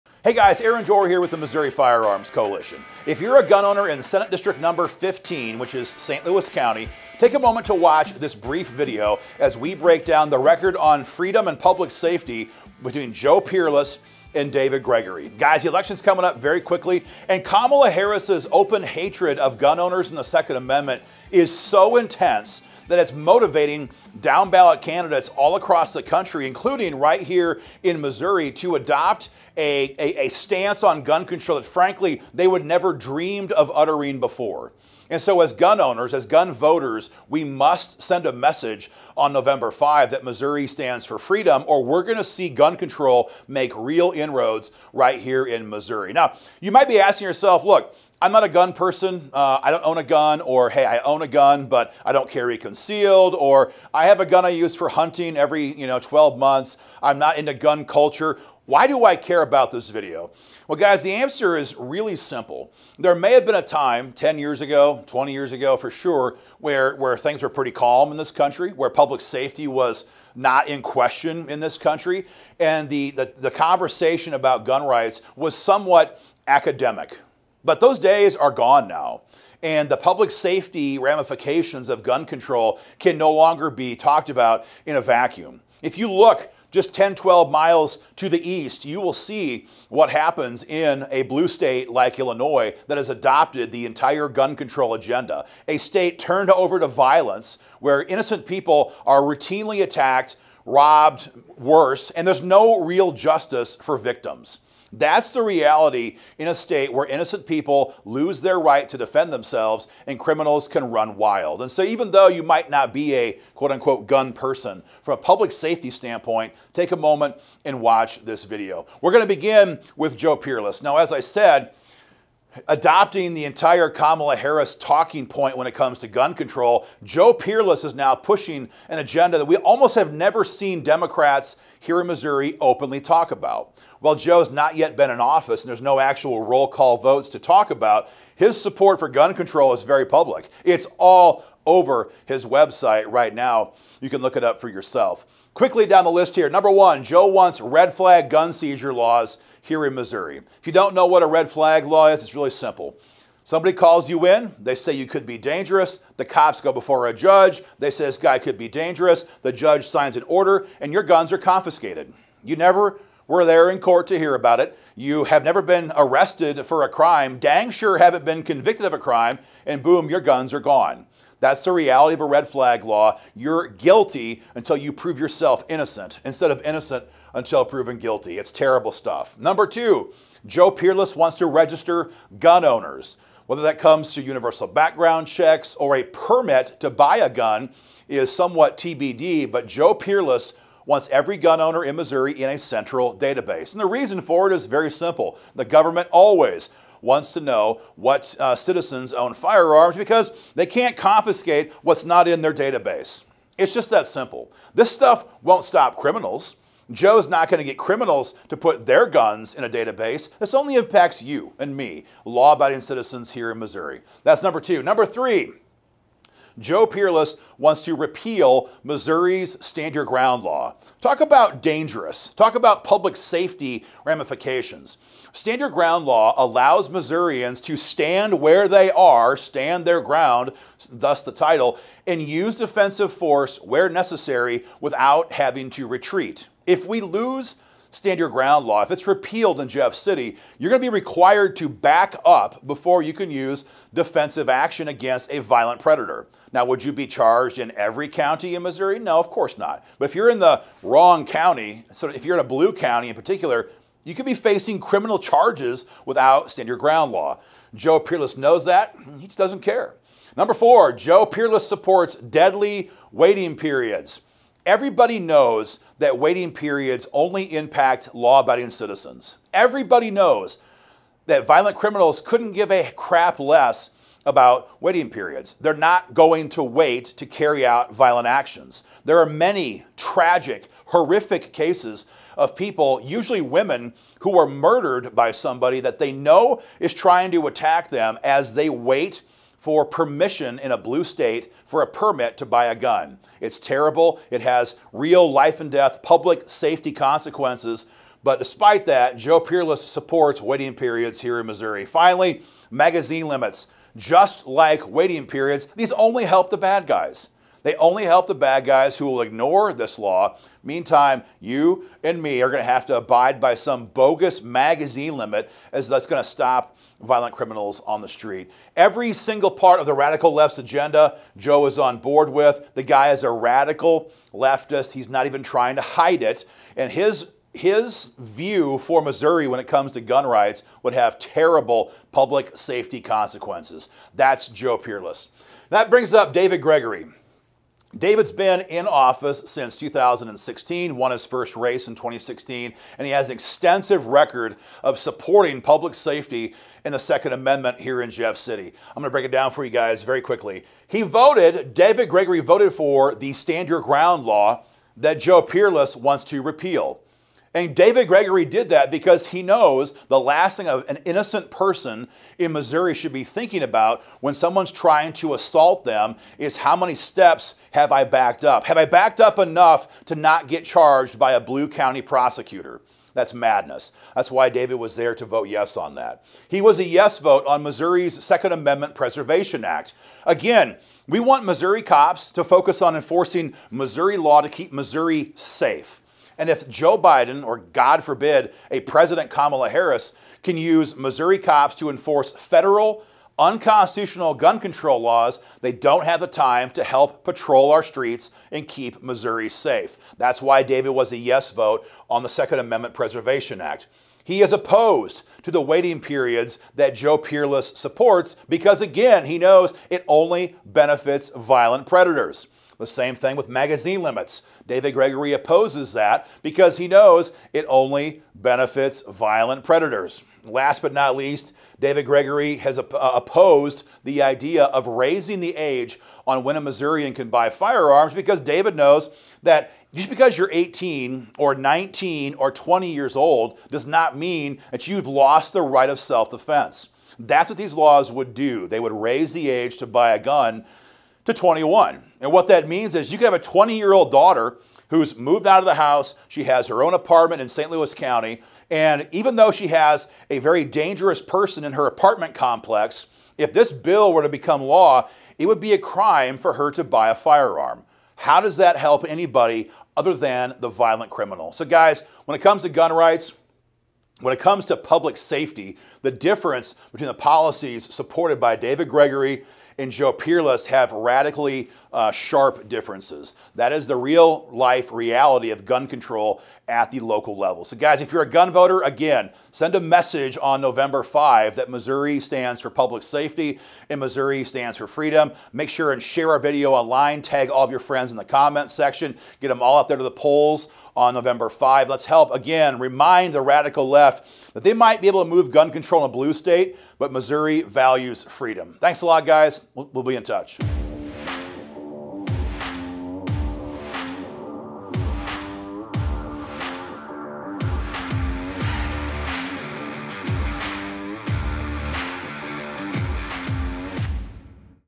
Gun Vote 2024: Missouri SD-15 (St. Louis County)
❗ ❗ Attention Gun Owners in Senate District 15! ❗ ❗

Who will stand and fight for gun owners – David Gregory or Joe Pereles?  Find out below!

Vote like YOUR gun rights depend on it on November 5th -- and share this with everyone you know!